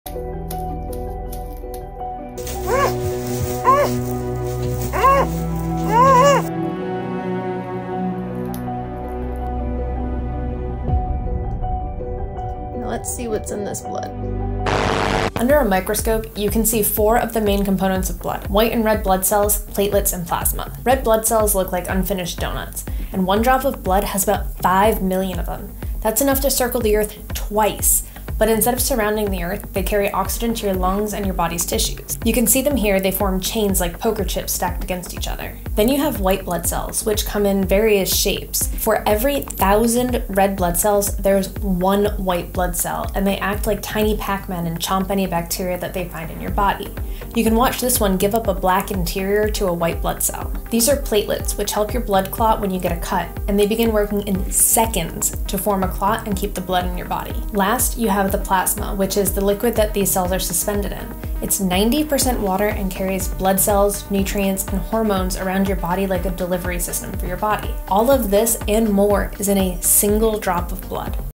Whats in Blood Under a Microscope?
Zoom in to learn what you can see under a microscope in a drop of blood.